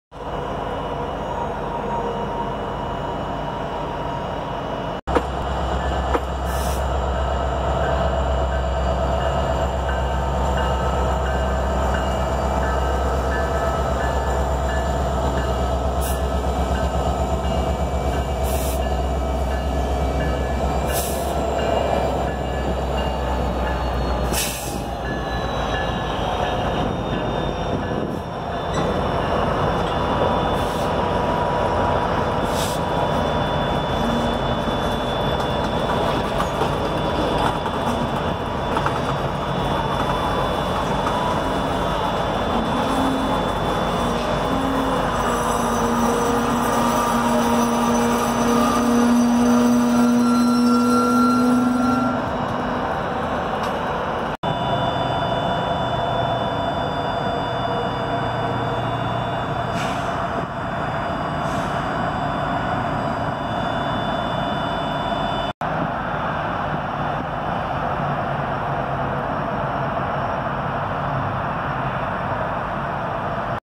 Norfolk Southern rolling up to a red light and a CSX track crew
Norfolk Southern rolling up to a red light and a CSX track crew at St. Bernard Soap.
SD70 # 7320 & Dash 9 # 9514.